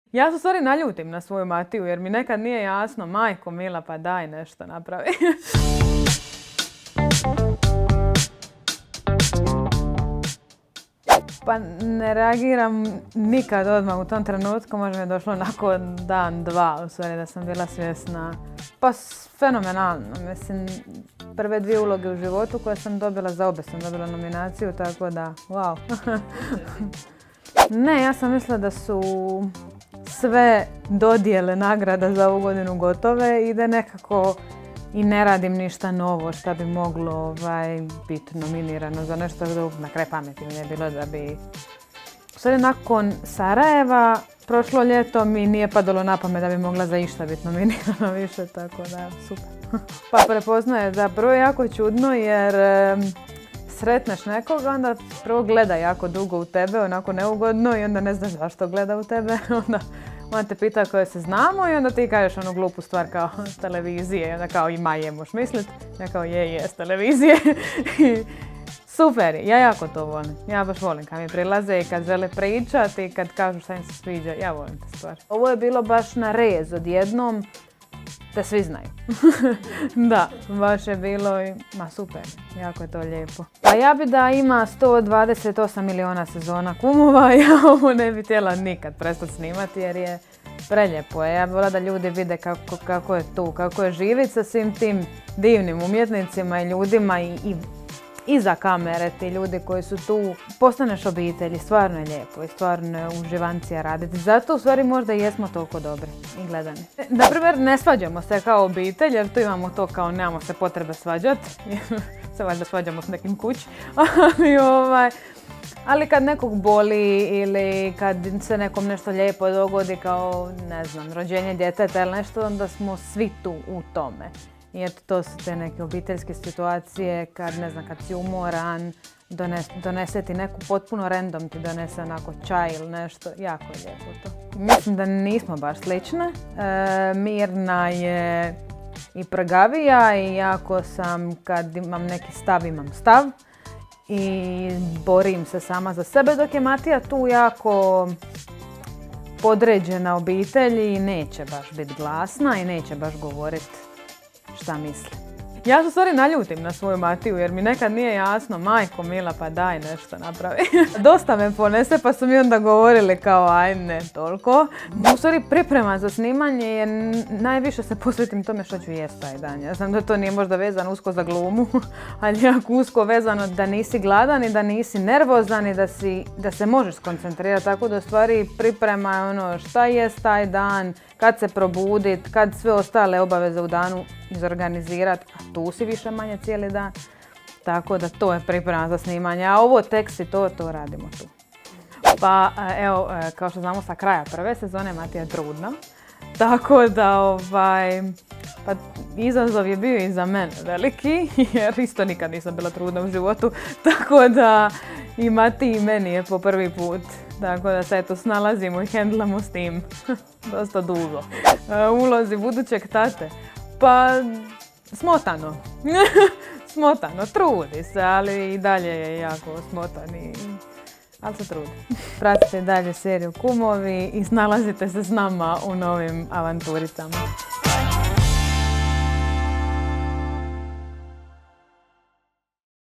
"Ja bih da ima 128 milijuna sezona Kumova" – Mirna Mihelčić najavila novu sezonu! | Nova Buzz
Nova sezona popularne humoristične serije Kumovi na male ekrane vraća omiljene likove iz Zaglava, pitoresknog mjesta u Dalmatinskoj zagori, a donosi i neke nove koji će svakako pomutiti planove starih mještana. U redovima Macana glumica Mirna Mihelčić utjelovila je Matiju s kojom je ostvarila veliku popularnost. Odjednom su je, kako sama kaže, svi znali, a to je prepoznala i struka. Nominirana je za Večernjakovu ružu u kategoriji novo lice.

Kako je reagirala na nominaciju, je li ju očekivala, koliko sličnosti ima s likom Matije i mnoge druge zanimljivosti otkrila nam je u novom videu. Otkrila je i što nas očekuje u novoj sezoni Kumova koja počinje uskoro na Novoj TV.